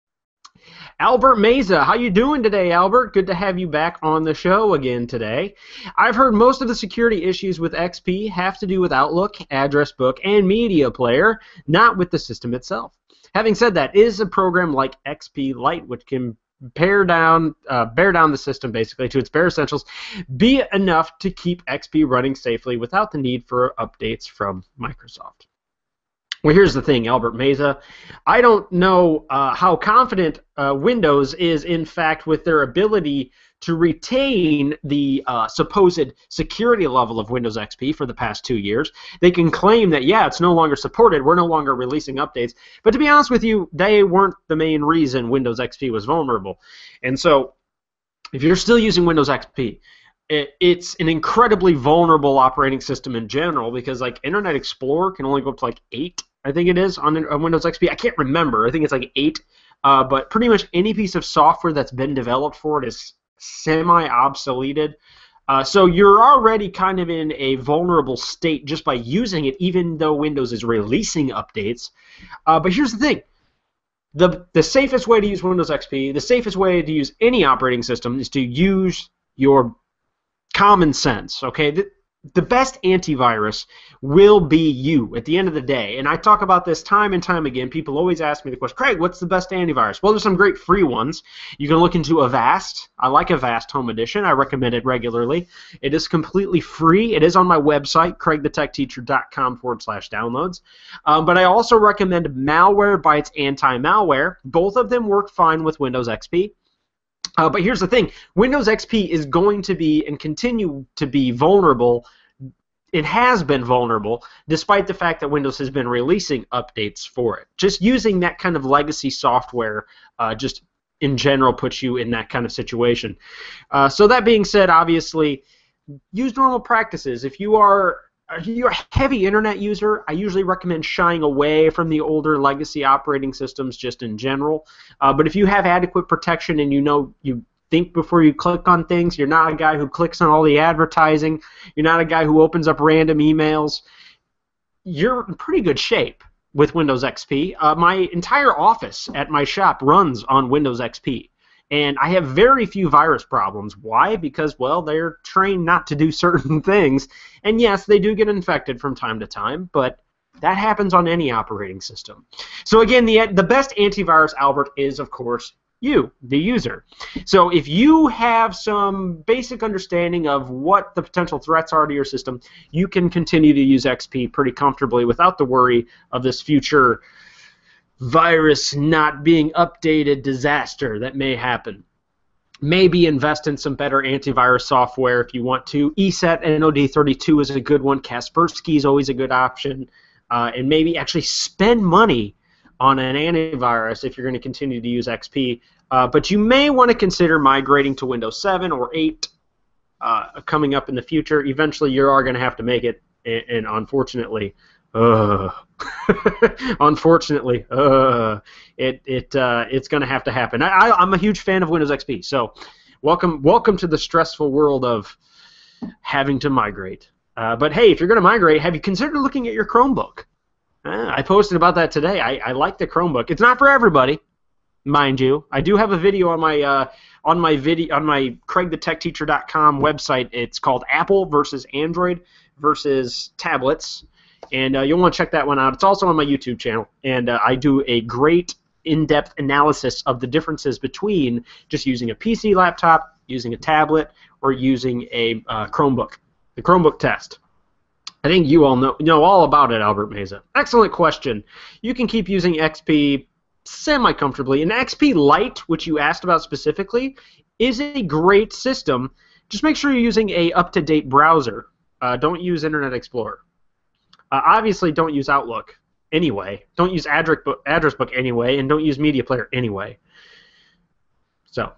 Windows XP Support End Date
The Windows XP Support End Date is April 8th 2014, should you still use it?